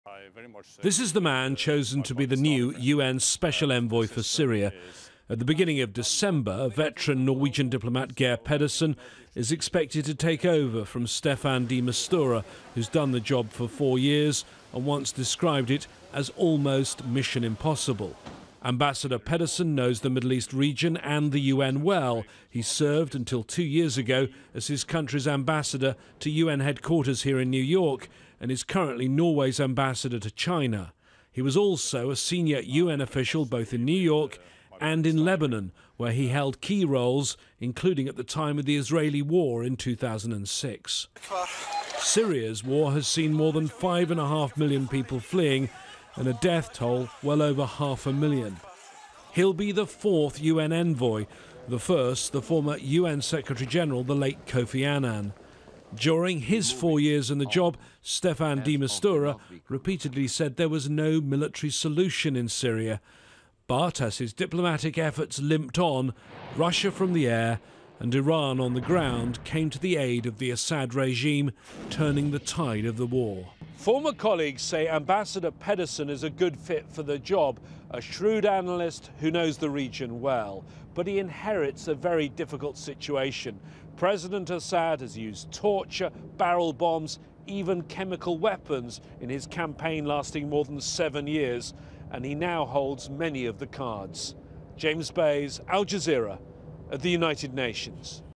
🇺🇳 UN secretary-general to appoint Geir Peders as Syria envoy | Al Jazeera English
Norwegian Diplomat Geir Pedersen is set to take over as the United Nations Special Envoy for Syria.
He has the unenviable task of leading negotiations to end the complex seven year war.
His predecessor, Staffan de Mistura, announced he was stepping down earlier this month.

Al Jazeera's James Bays reports from the United Nations.